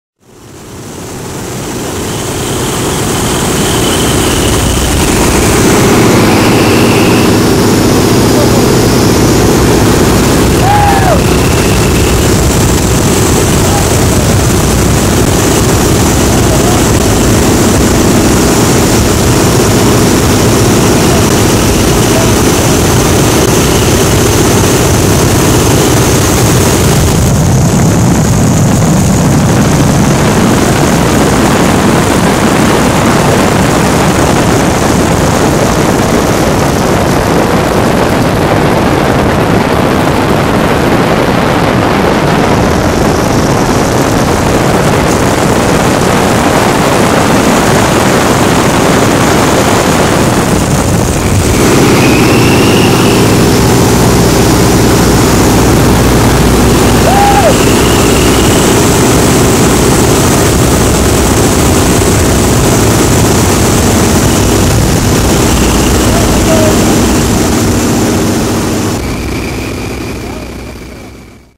Stearmen Aerobatics in the Front Seat
Did a glider tow, glider recovery and aerobatics in this Stearmen.  Amazing experience.  Video was taken in may of 2000